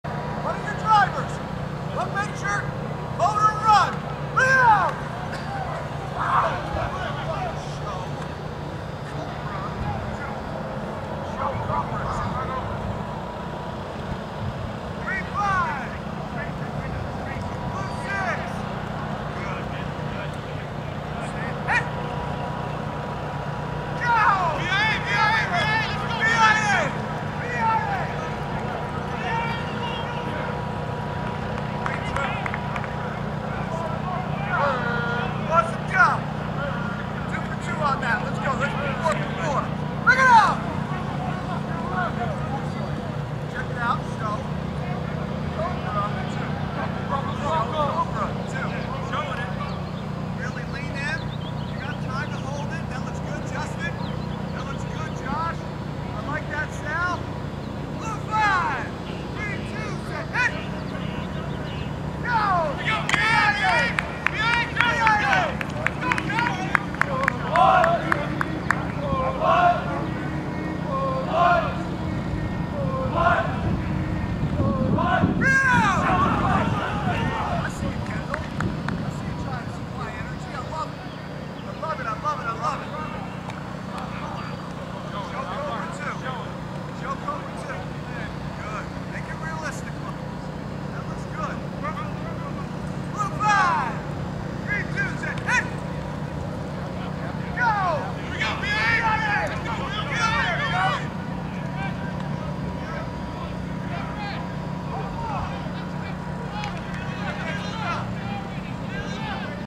Notre Dame Spring Practice: Day 9 - Defense
Clips of Notre Dame's ninth day of spring practice - defense.